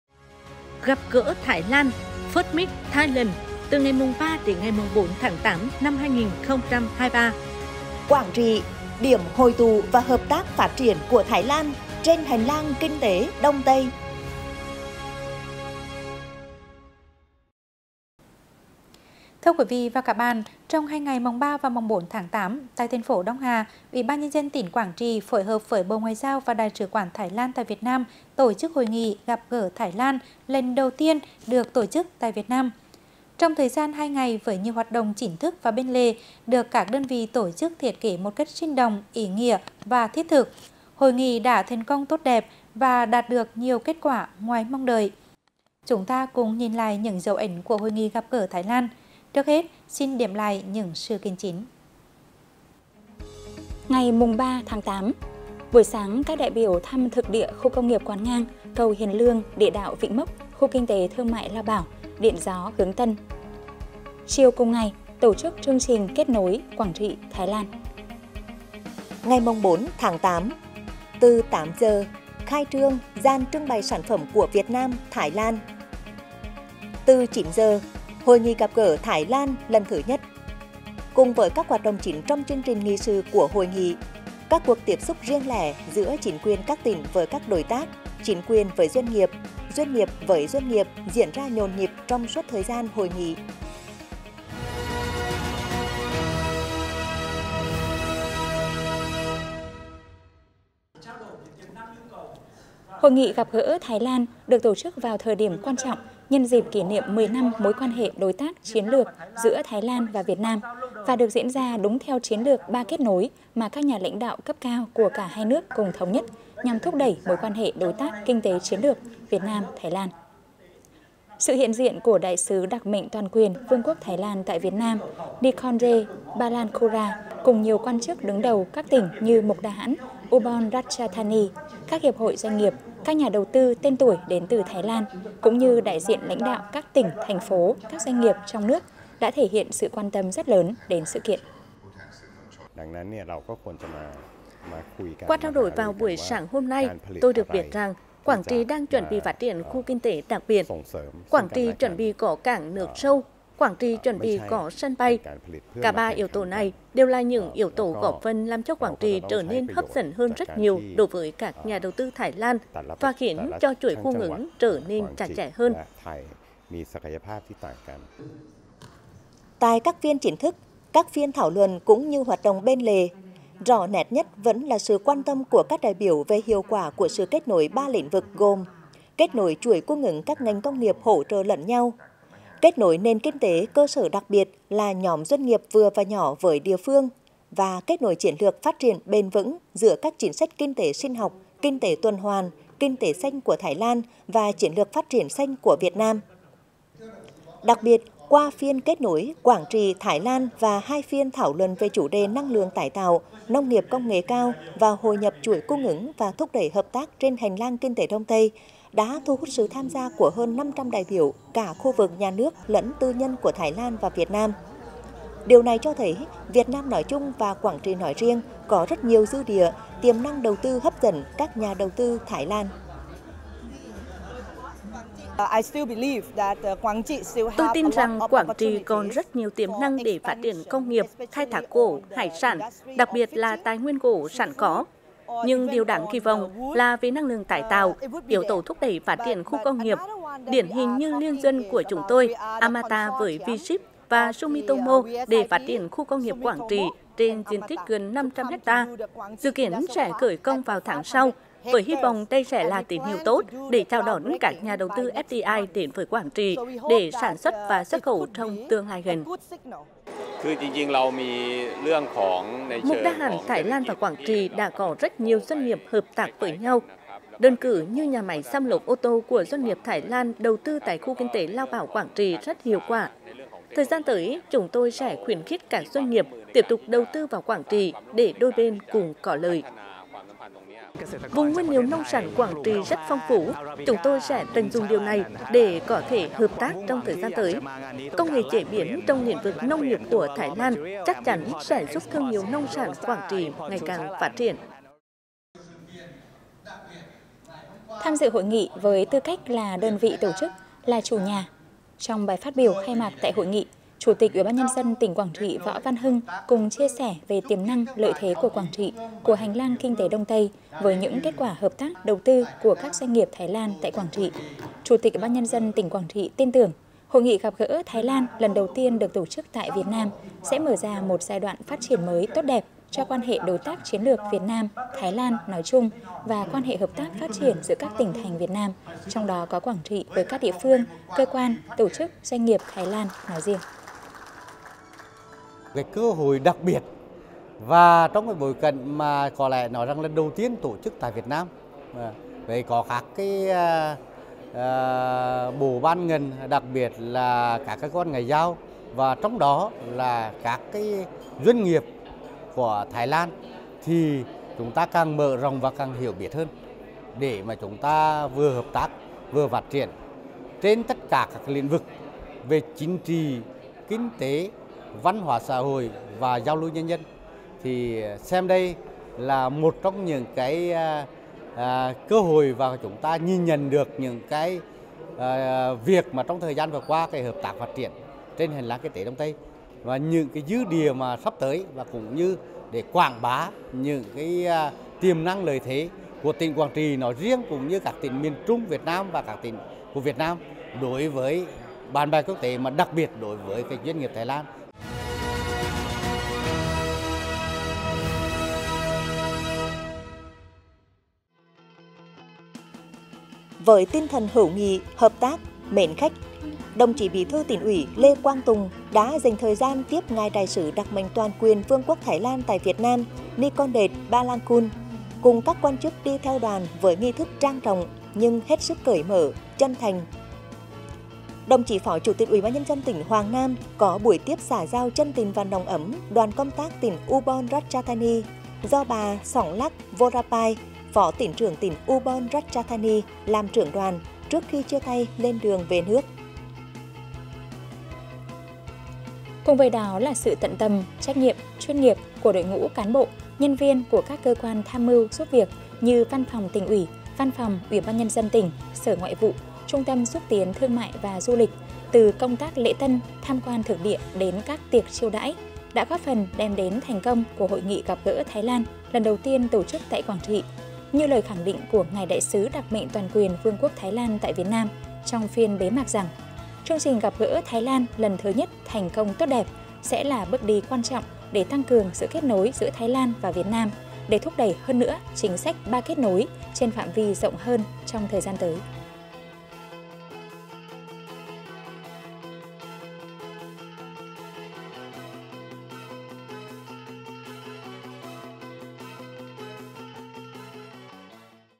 DẤU ẤN CHƯƠNG TRÌNH GẶP GỠ THÁI LAN - Truyền Hình Quảng Trị
Trong 02 ngày 03 - 04/8/2023 tại TP Đông Hà, UBND tỉnh Quảng Trị phối hợp với Bộ Ngoại giao và Đại sứ quán Thái Lan tại Việt Nam tổ chức Hội nghị “Gặp gỡ Thái Lan” lần đầu tiên được tổ chức tại Việt Nam. Hội nghị đã thành công tốt đẹp và đạt được nhiều kết quả ngoài mong đợi. Chúng ta cùng nhìn lại những dấu ấn của Hội nghị gặp gỡ Thái Lan.